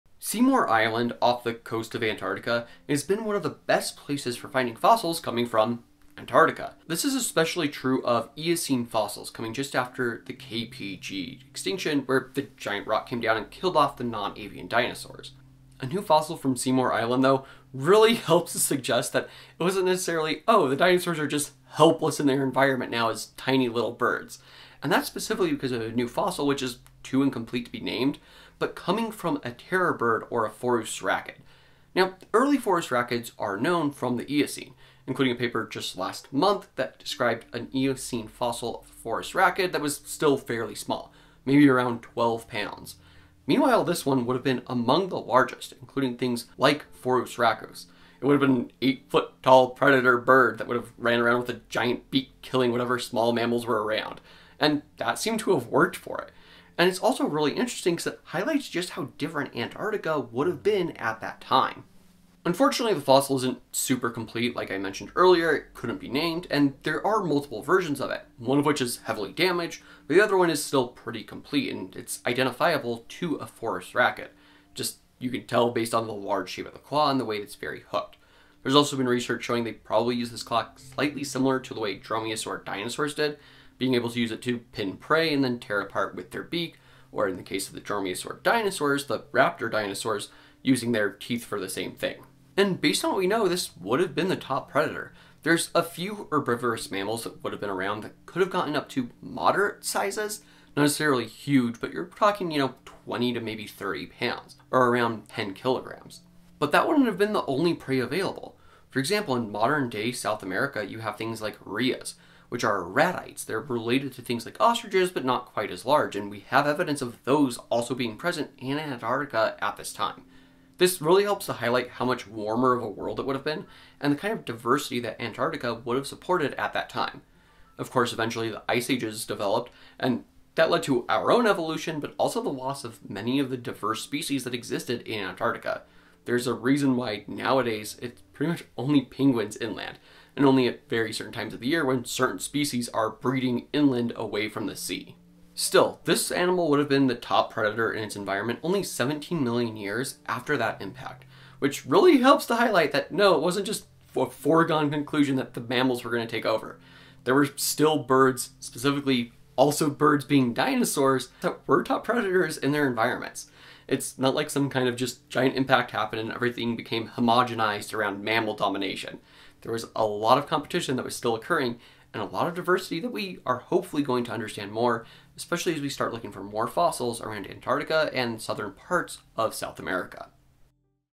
New Terror Bird from Antarctica
New fossils from Seymour Island, Antarctica have been shown to belong to probably the top predator of the continent during the Eocene, a terror bird, standing nearly 9 feet in height, and with curved claws for holding down prey.  It really wasn't an immediate jump into the age of mammals, because the dinosaurs were in some cases still holding their own very well.